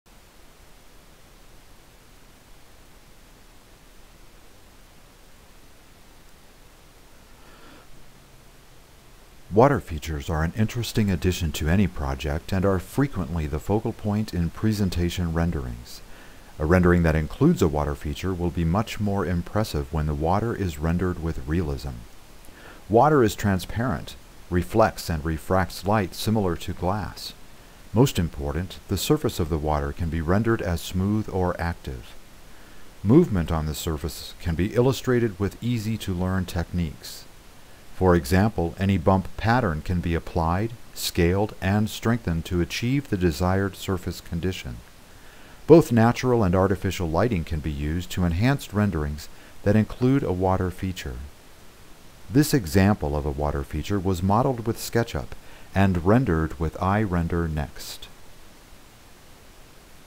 SketchUp Rendering - Water Features
When a water feature is added to a project it will frequently become the focal point in presentation images.  This video illustrates an easy to learn and use technique to explore rendering different surface conditions on any water feature.